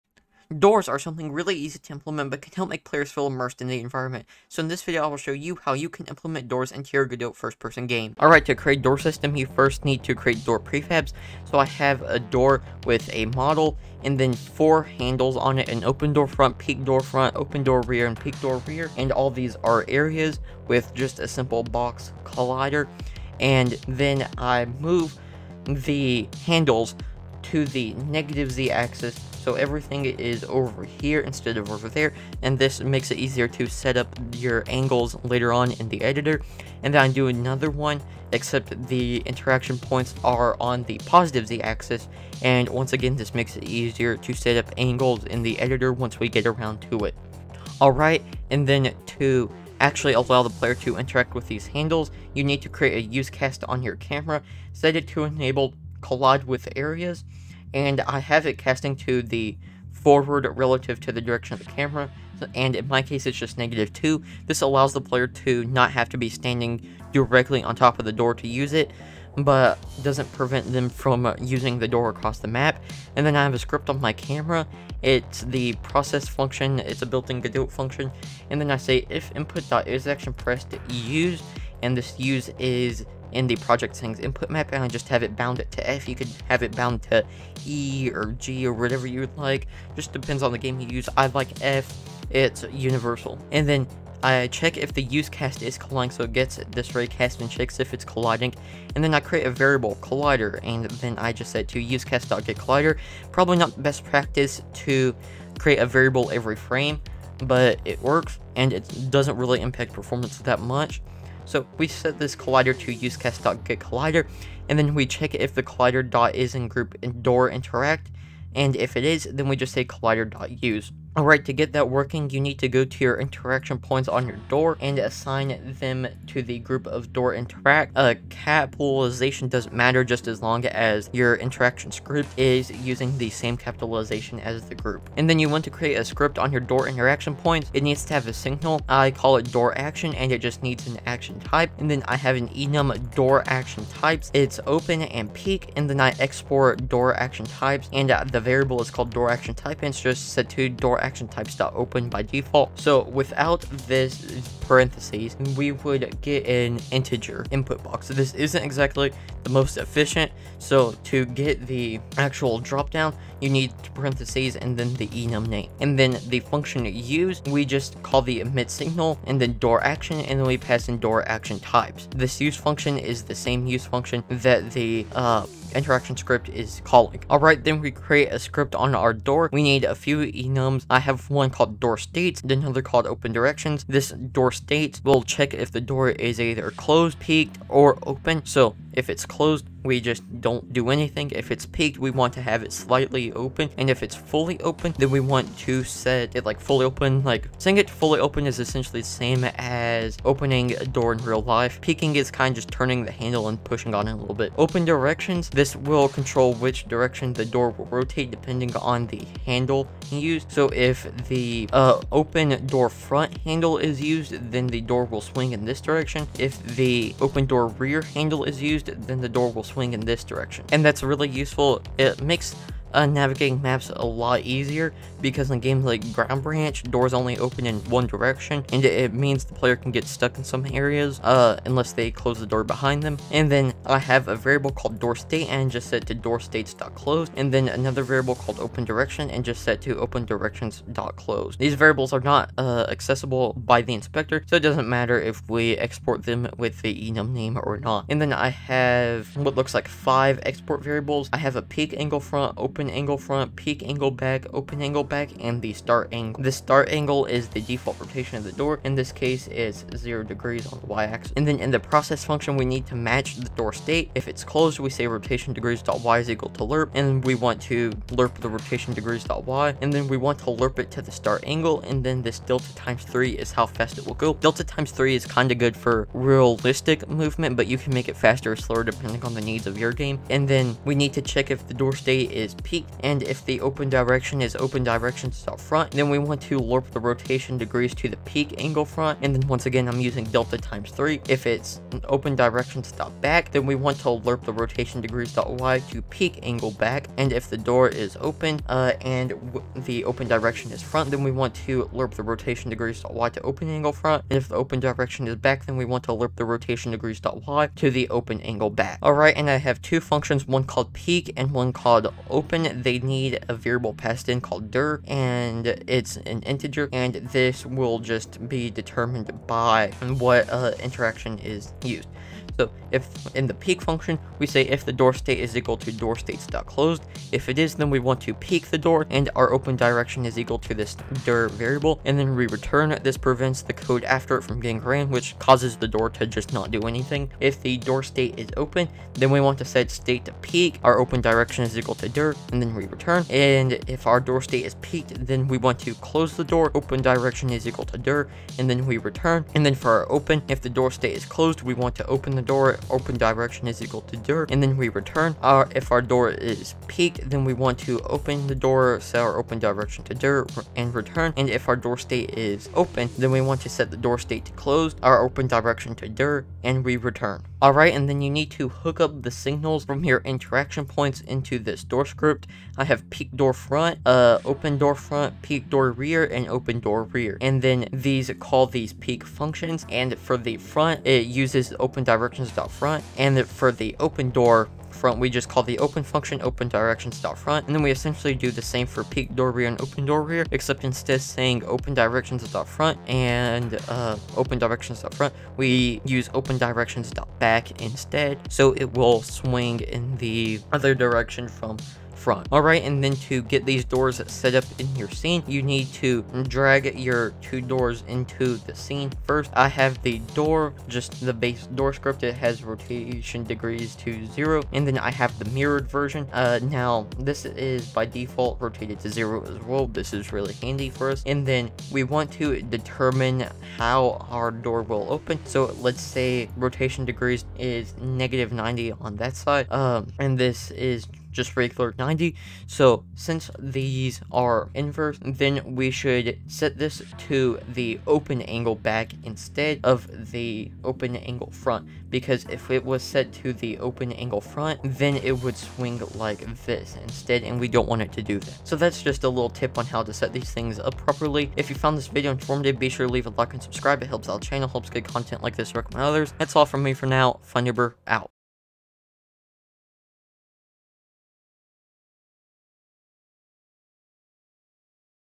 make environments INTERACTIVE with DOORS in Godot!
In this video I will show you how you can utilize signals and state machines to make a door system in the Godot Engine.

Doors are a really easy and simple way to introduce interactive environments into any game. This video will show you how you can utilize this simple system to allow players to interact with the environment.